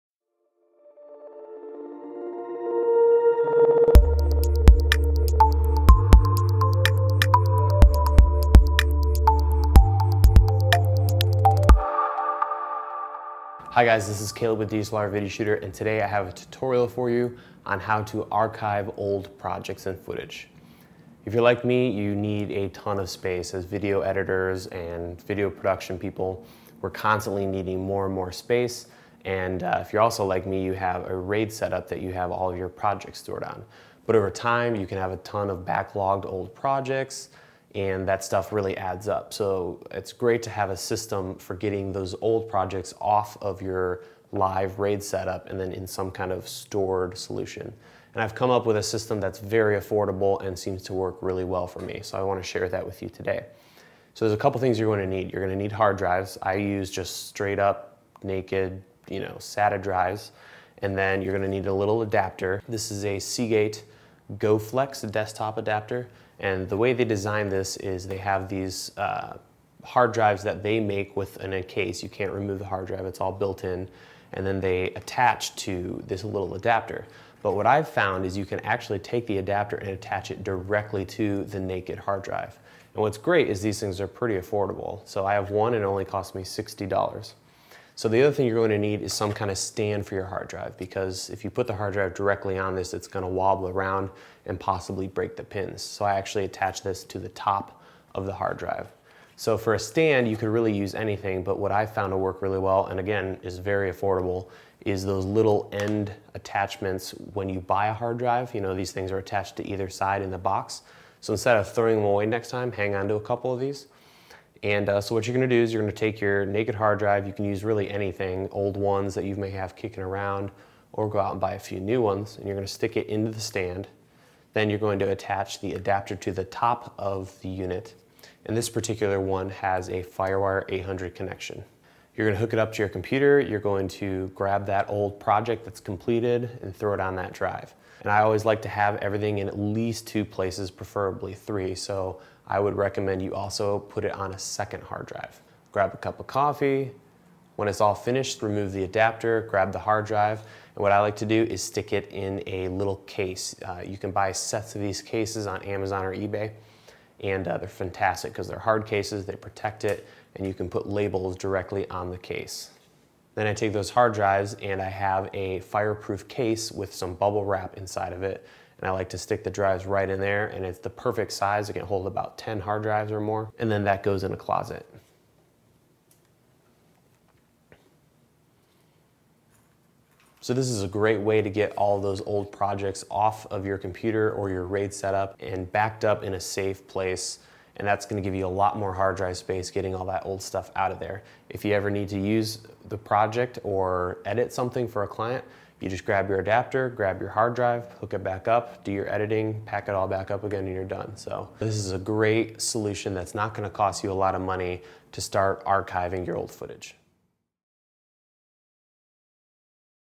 Tutorial: Affordably Archiving And Backing Up Old/Finished Projects
All video editors and content creators need hard drive space... And LOTS of it. Here is a great solution for getting old/finished projects off of your computer drives and onto a safe, backed up drive on the cheap.

WHAT YOU NEED:

USB 3.0 Adapter: $13
Firewire 800+USB: $59
2x 1TB Drives: $100-125 (or Free if you have old ones)
Fireproof Safe: $25
(Optional): Hard drive cases

RESULT: Solid Backup Solution for $13-185.

UPDATE: A lot of people are commenting on how this is a bad idea for long-term storage (years). So yes, this isn't the greatest way to store projects for extended periods of time. But this is still a great way to move data around and store projects. It has worked for me and my clients for several years. Just always make sure you have at least 2 or 3 copies!!!